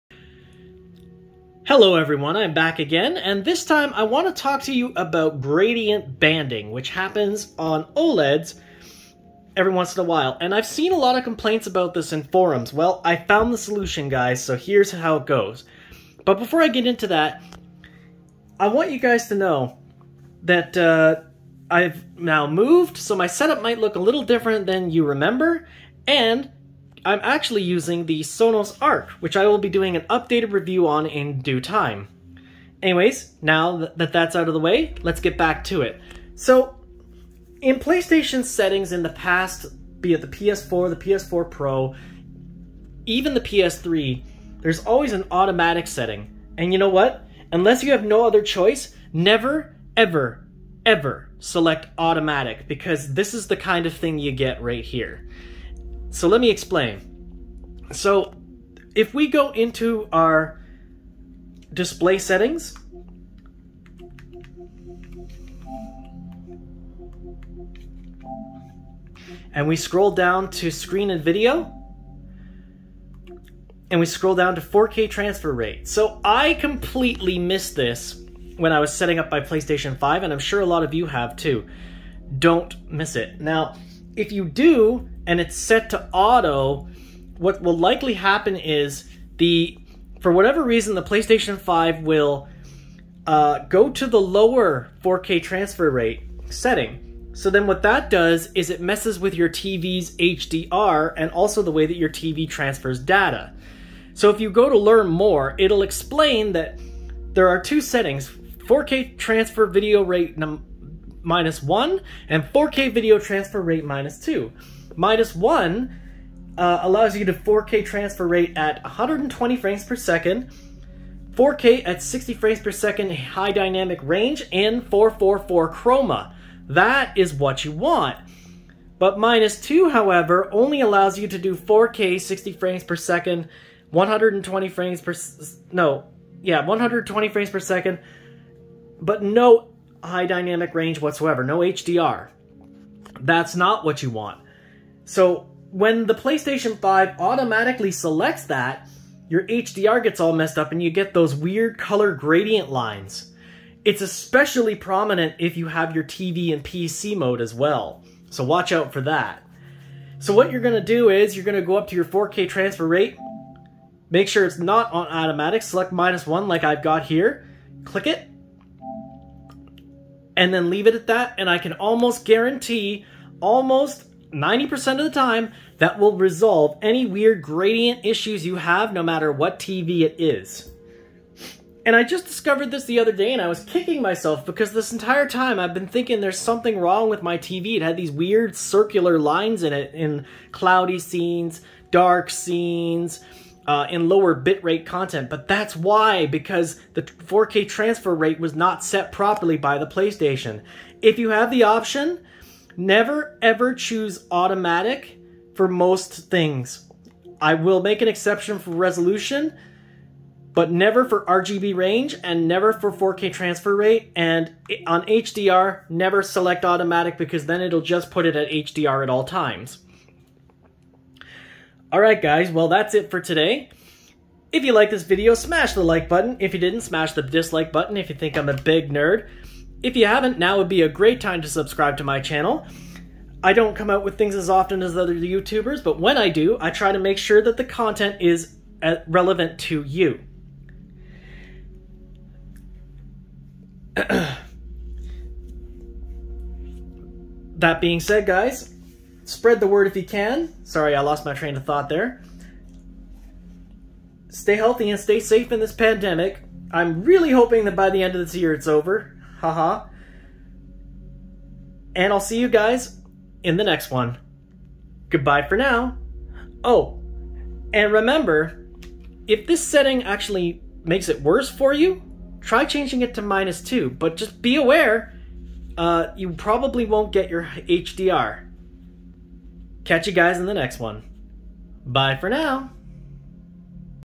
PS5 color gradient banding fix
How to fix those weird circular lines that appear in dark or cloudy scenes.